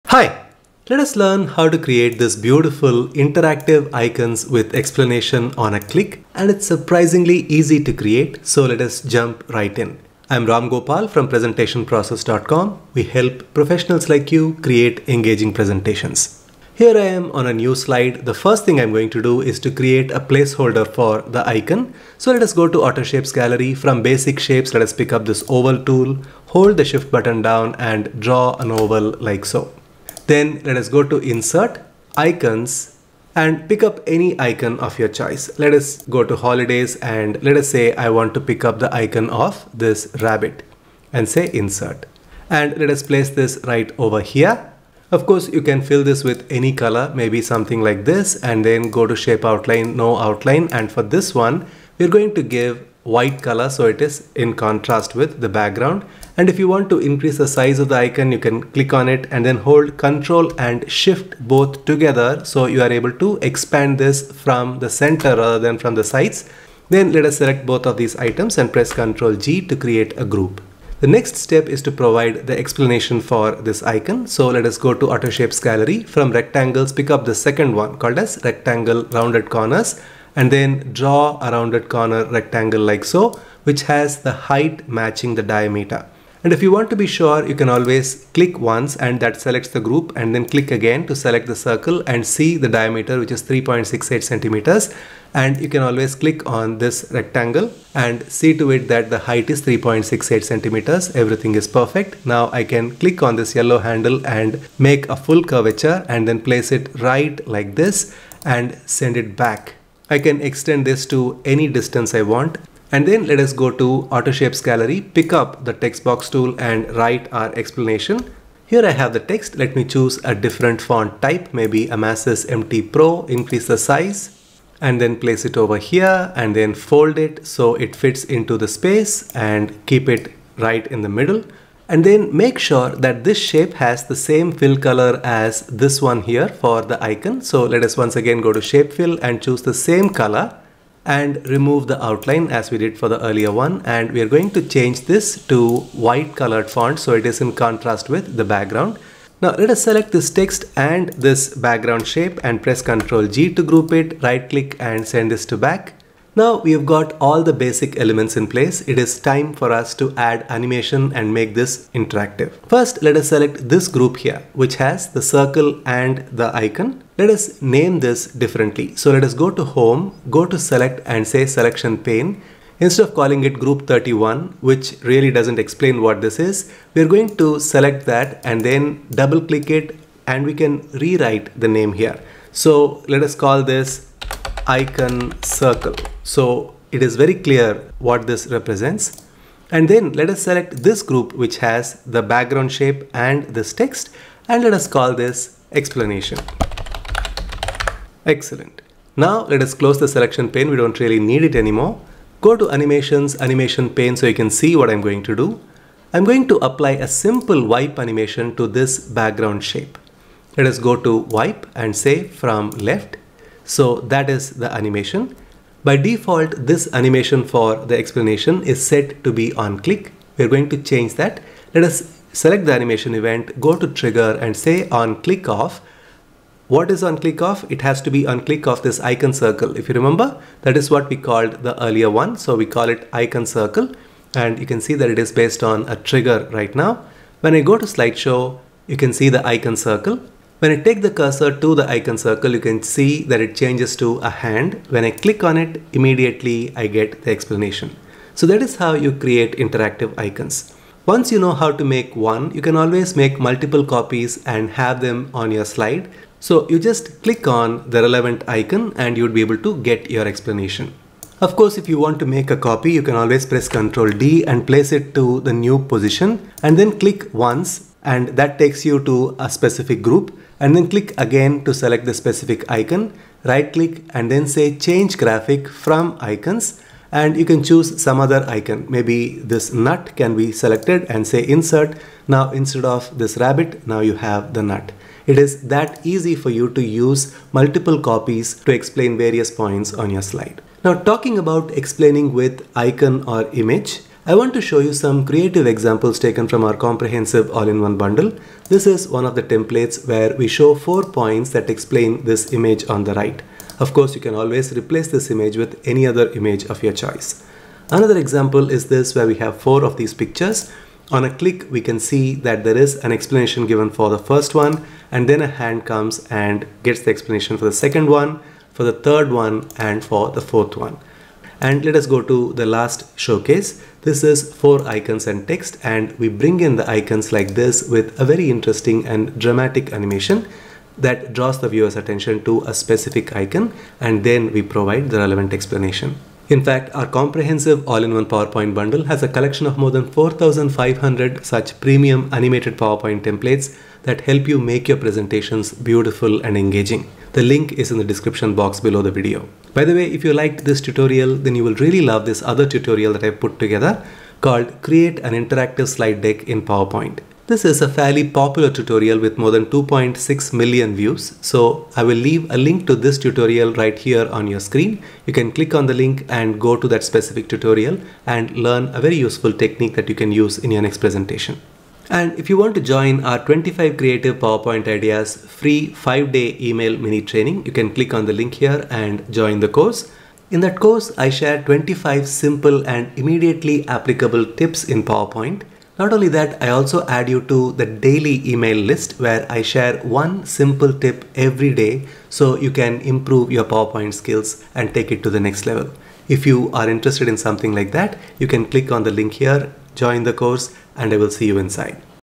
Interactive Explanation in PowerPoint - Easy for Beginners
Create engaging presentations with Interactive Explanation in PowerPoint. This is easy even for Beginners. Comprehensive All In One PowerPoint TEMPLATES Bundle 2.0

Contents of this video:
00:00 Preview
00:19 Create Placeholder & Icon
01:13 Add Explanation Text
02:40 Make Interactive
04:11 Duplicate as required
04:52 Creative Ways to present lists